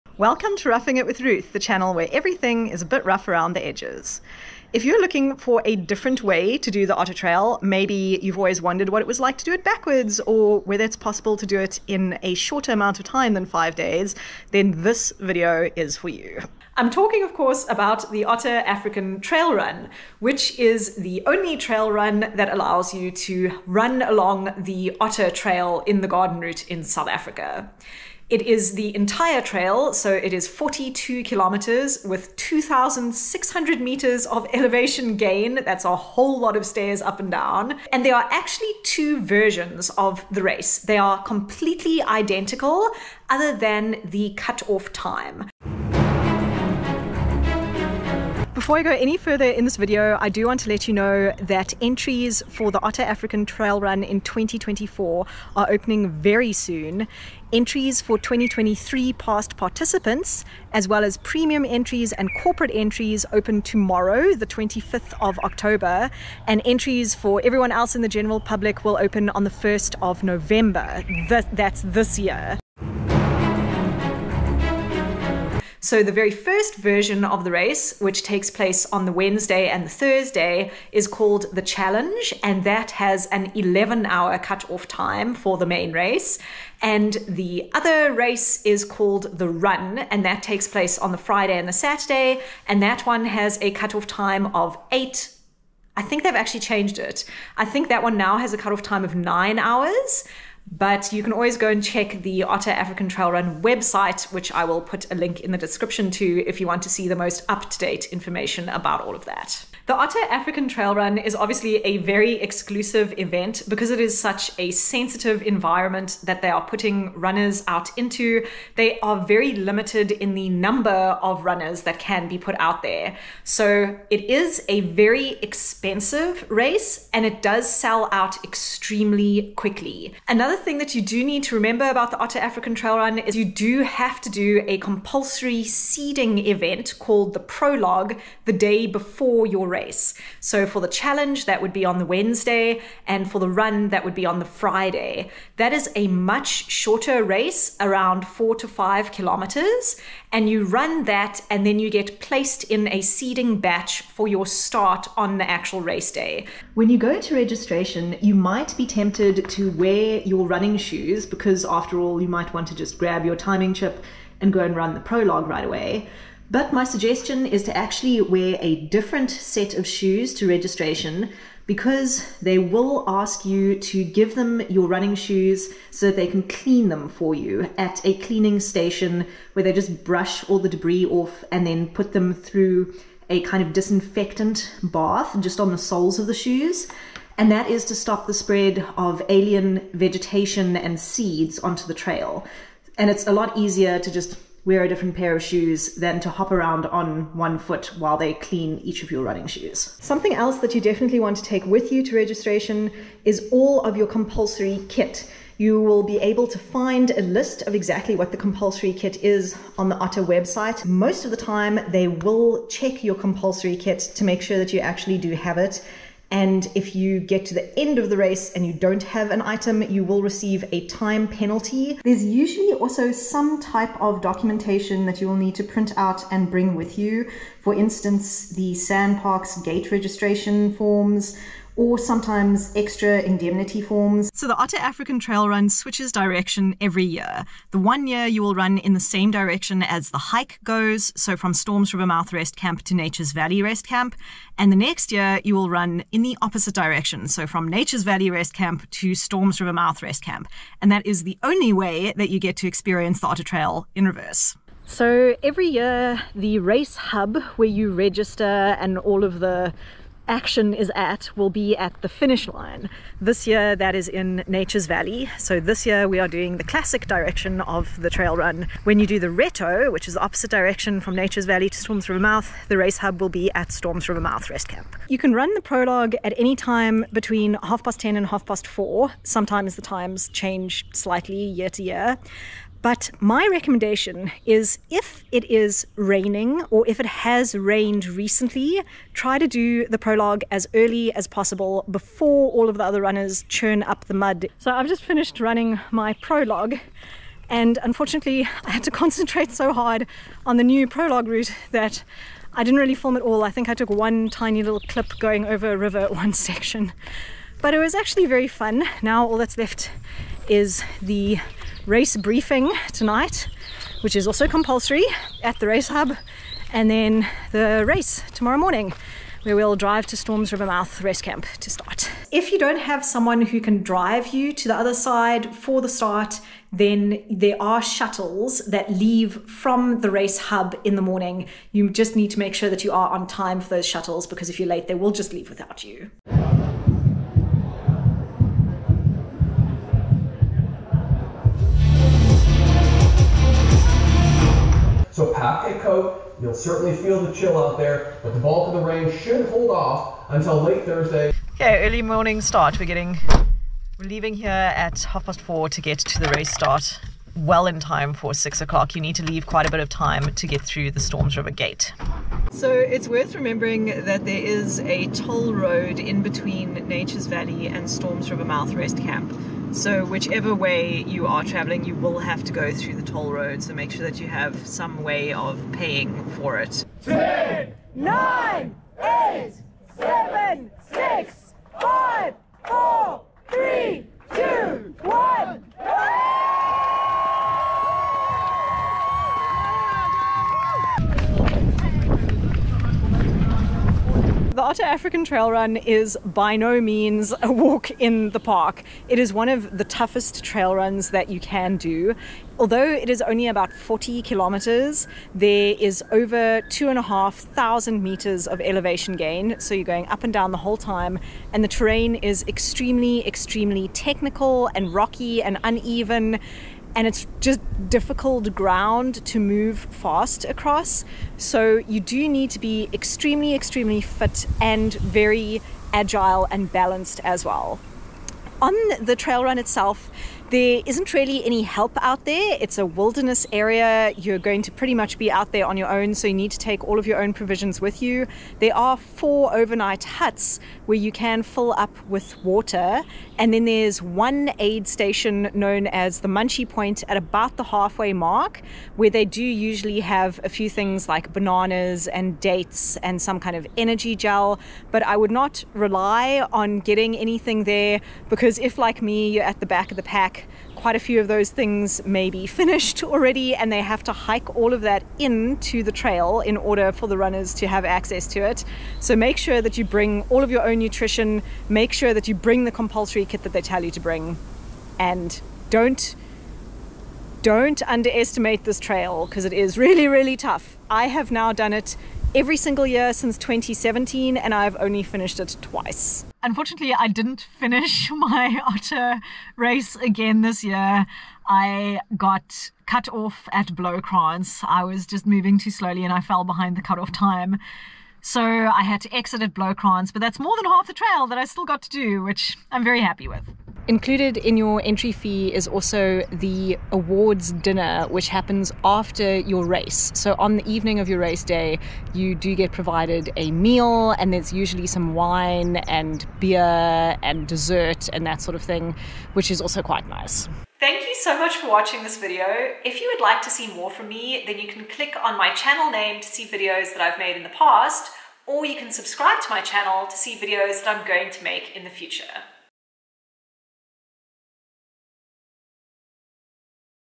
Can you do The Otter in ONE DAY? | Everything you need to know about The Otter African Trail Run
The Otter African Trail Run is also known as The Grail of Trail. Here is all the information you need to enter, prepare and organize for the race!